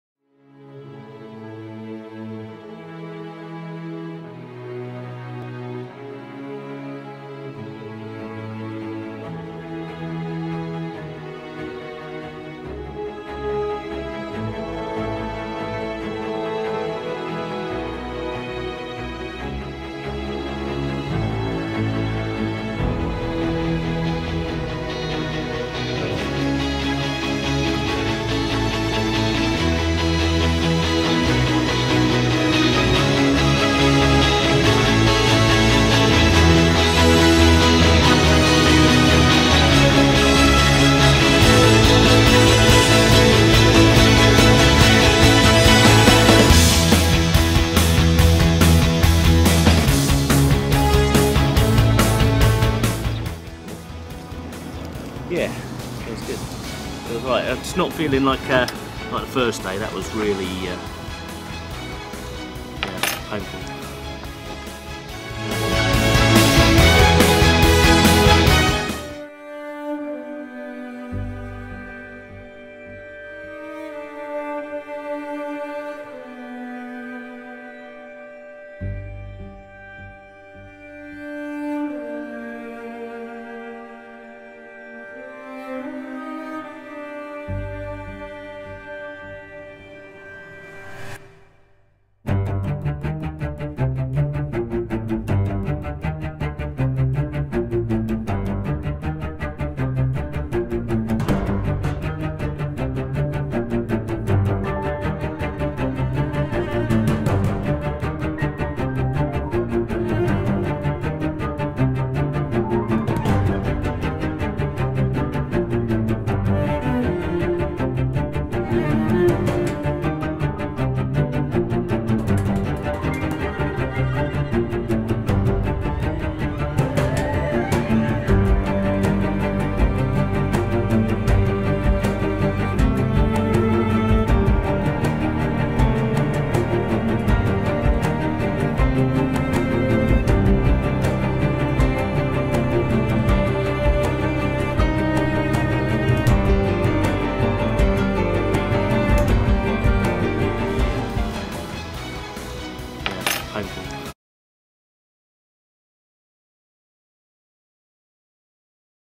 Quading it up lands end to John o groats on quad bikes
An epic lejog trip on quad bikes to raise money for cancer research ended in tragedy. Anthony Copeland and Chris Thompson cover over 1100 miles in 5 days.